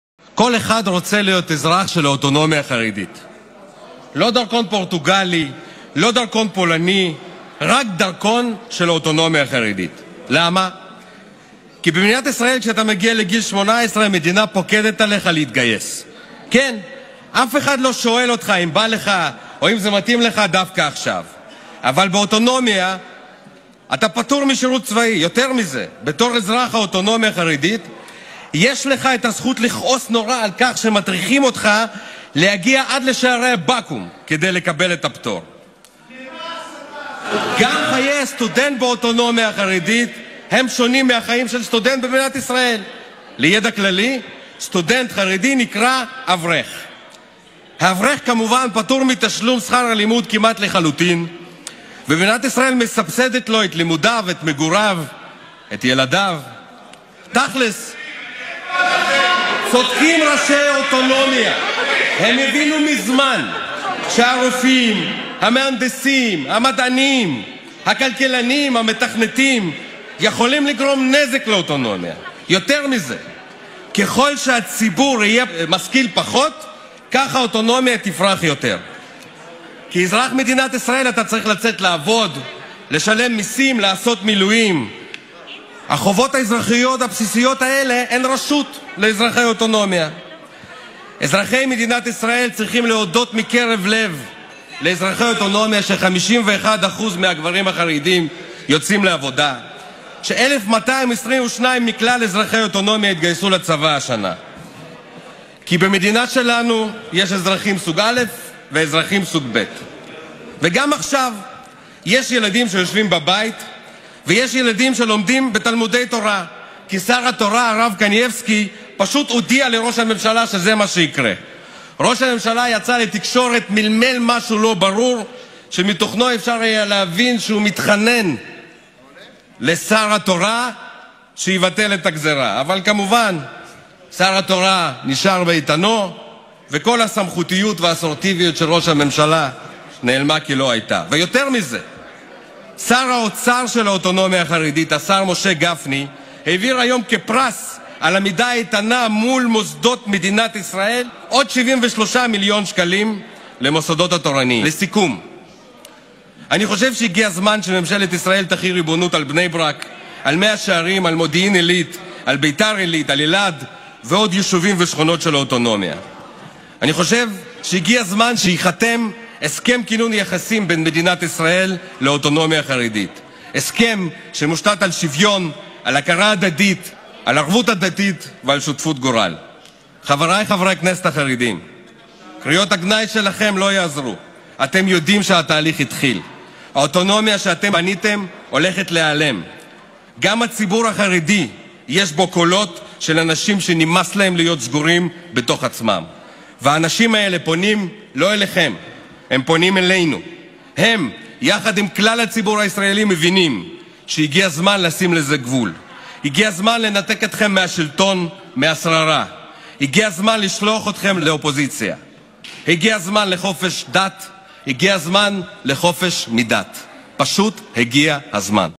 Новости Израиля.А.Кушнир: Распространить израильский суверенитет на Ультраортодоксальную автономию.
Кушнир Алекс:
Распространить израильский суверенитет на Ультраортодоксальную автономию!